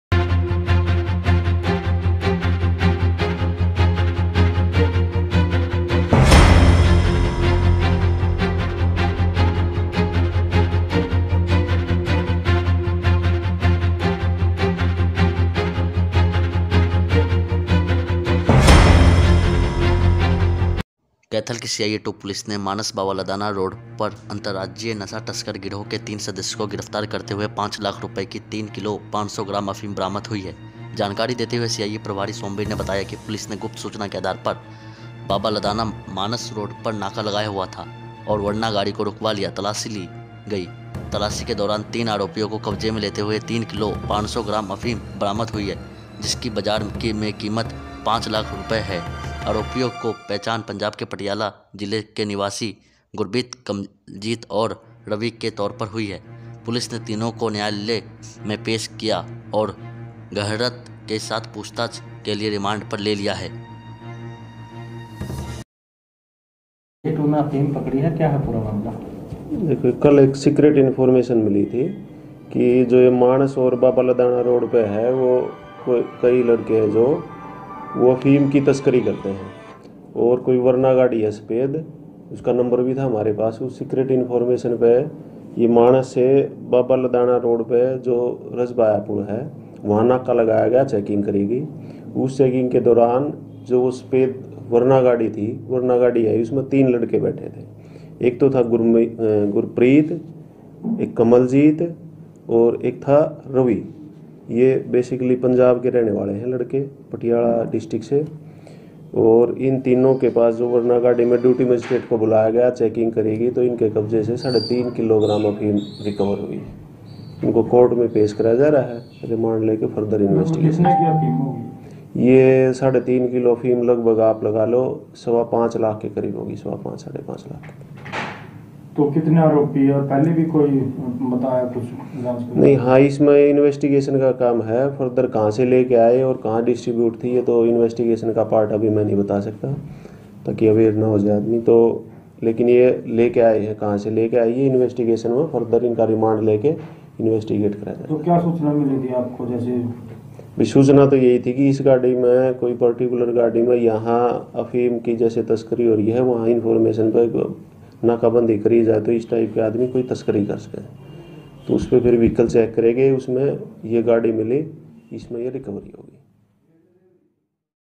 अंतरराज्यीय गिरोह के 3 सदस्यों को नशे की बड़ी खेप के साथ किया गिरफ्तार
कैथल न्यूज़
कैथल की सीआईए पुलिस ने अंतरराज्यीय गिरोह के 3 सदस्यों को नशे की बड़ी खेप के साथ किया गिरफ्तार,
बाबालदाना -मानस रोड पर नाका लगाकर वरना गाड़ी से किया गिरफ्तार

कब्जे से 5 लाख मूल्य की 3 किलो 500 ग्राम अफीम हुई बरामद

कैथल की सीआईए-2 पुलिस ने मानस-बाबालदाना रोड से अंतरराज्यीय नशा तस्कर गिरोह के 3 सदस्यों को गिरफ्तार करते हुए 5 लाख रुपये की 3 किलो 500 ग्राम अफीम बरामद हुई है जानकारी देते हुए सीआईए प्रभारी सोमवीर ने बताया कि पुलिस गुप्त सूचना के आधार पर बाबा लाना मानस रोड पर नाका लगाया हुआ था और वरना गाड़ी को रुकवा कर तलाशी ली गई तलाशी के दौरान तीन आरोपियों के कब्जे से 3 किलो 500 ग्राम अफीम बरामद हुई जिसकी बाजार में कीमत लगभग ₹500000 है आरोपियों की पहचान पंजाब के पटियाला जिले के निवासी गुरप्रीत कमलजीत और रवि के तौर पर हुई है पुलिस ने तीनों को न्यायालय में पेश किया और गहनता से पूछताछ के लिए रिमांड हासिल किया है